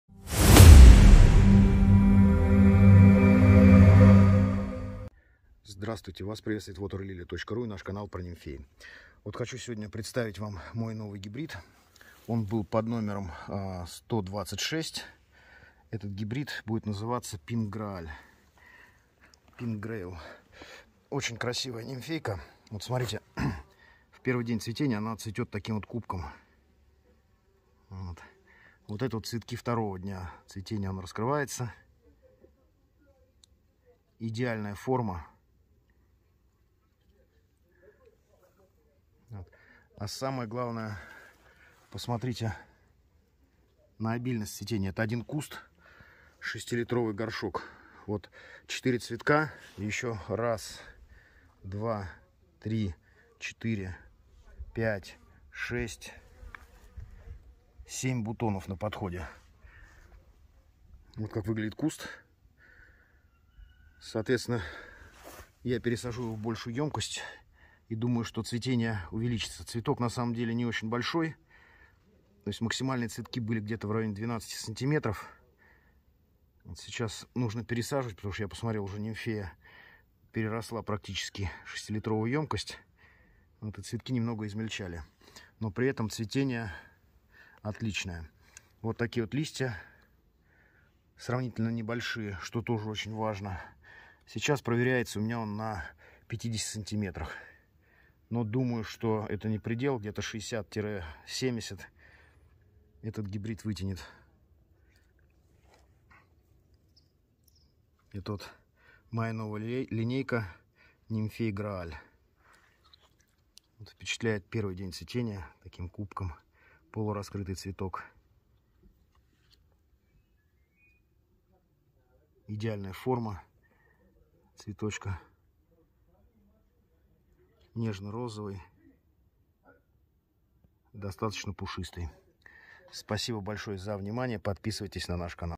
Нимфея WLK Pink Grail (Нимфея Розовый Грааль), селекции Хох Константина, питомник Waterlilia.ru
Нимфея WLK Pink Grail (кувшинка WLK Розовый Грааль), этот гибрид выведен в питомнике WaterLilia.ru в 2020 году, селекционер Хох Константин, Россия.

Отличительной особенностью всех гибридов нимфей, выведенных в питомнике WaterLilia.ru будет аббревиатура WLK (это означает, что нимфея селекции WaterLilia.ru и гибридезатор Konstantin Khokh)

Grail – это новая линейка красивейших гибридов моей селекции, будут различные вариации цветов и форм цветка!

Цветок нимфеи WLK Pink Grail нежно розового цвета, около 12 см. в диаметре идеальной формы и симетрии. Тычинки у нимфеи желтые. Форма цветка чашевидная, цветет на поверхности воды в виде кубка. На второй и третий день раскрывается все больше и больше.  Цветет очень обильно на 2 год, когда хорошо разрастется!
Листья у кувшинки WLK Pink Grail почти круглой формы около 15 -18 см. в диаметре. Нижний разрез у листьев закрытый. Молодые листья у нимфеи коричневого цвета, со временем листья приобретают зеленый цвет.
Оптимальная глубина посадки данной нимфеи 50 – 60 см. Первоначальную посадку рекомендуем в емкость 5 литров и более. Разрастается хорошо. Нимфея Розовый Грааль отлично зимует в условиях Подмосковья. Подходит для средних водоемов.

Наше мнение о нимфеи WLK Pink Grail
Эксклюзивная новинка от селекционера Константина Хох (питомник WaterLilia.ru). Кувшинка Розовый Грааль очень оригинально смотрится в водоеме за счет своего уникального нежно розового цвета, формы цветка в виде кубка и обильности цветения. Также нимфея имеет не большие по размеру листья и при этом очень активно цветет, что очень актуально для небольших водоемов.
Нимфея WLK Pink Grail будет всегда в центре внимания Вашего водоема – это находка у кого не большой по размерам пруд и для тех кто любит нежные тона.